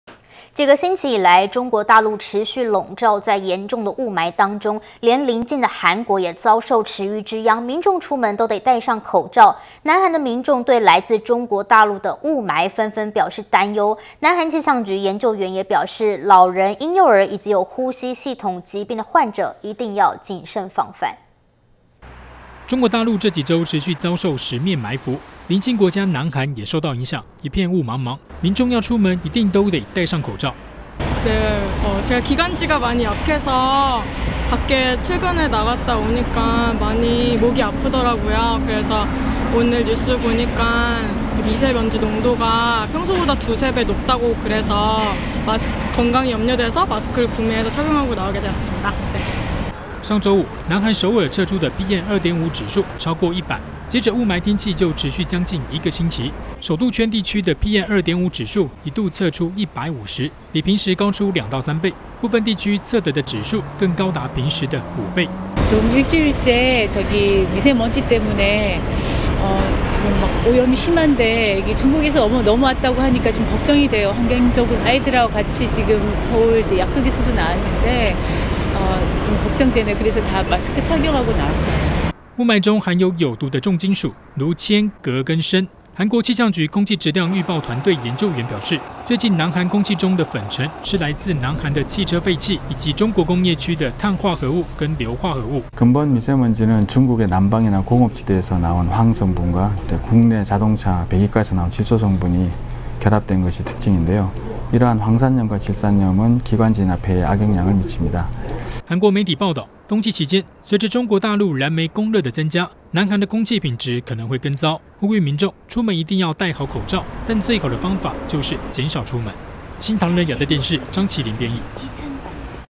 【中國真相最新新聞】中國霧霾飄鄰國 南韓民眾警戒
【中國真相新聞】(China Breaking News)南韓民眾對來自中國大陸的霧霾紛紛表示擔憂。南韓氣象局研究員也表示，老人、嬰幼兒，以及有呼吸系統疾病的患者一定要謹慎防範。

中國大陸這幾週持續遭受「十面霾伏」，鄰近國家南韓也受到影響，一片霧茫茫。民眾要出門，一定都得戴上口罩。

韓國民眾：「我的支氣管不太好，當我在外面的時候，我的喉嚨感覺很糟糕。我今天看新聞說，粉塵濃度比平時高兩三倍，我很擔心我的健康狀況。」

上週五，南韓首爾測出的PM2.5指數超過100，接著霧霾天氣便持續將近一個星期，首都圈地區的PM2.5指數一度測出150，比平時高出2到3倍，部分地區測得的指數更高達平時的5倍。

韓國民眾：「這一星期空氣被污染的很嚴重，因為微細粉塵。我聽說它(這些微細粉塵)是從中國來的，所以我很擔心，因為那裡環境很不好。/我今天和我的小孩一起出來，我有點擔心，所以我們出門都戴口罩。」

霧霾中含有有毒的重金屬，如鉛、鎘和砷。韓國氣象局空氣質量預報團隊研究員表示，最近南韓空氣中的粉塵是來自韓國的汽車廢氣以及中國工業區的氮化合物和硫化合物。

韓國氣象局空氣質量預報團隊　高級研究員：「這些硫酸鹽和硝酸鹽對支氣管和肺部會產生不良影響。同時，四天以來積累的粉塵正在逐漸消失，但它仍然是在『小壞』的狀態，所以，兒童、老人和嬰幼兒，以及有呼吸系統疾病的患者一定要謹慎防範。」

韓國媒體報導，冬季期間，隨著中國大陸燃煤供熱的增加，南韓的空氣品質可能會更糟。呼籲民眾，出門一定要戴好口罩，但最好的方法就是減少出門。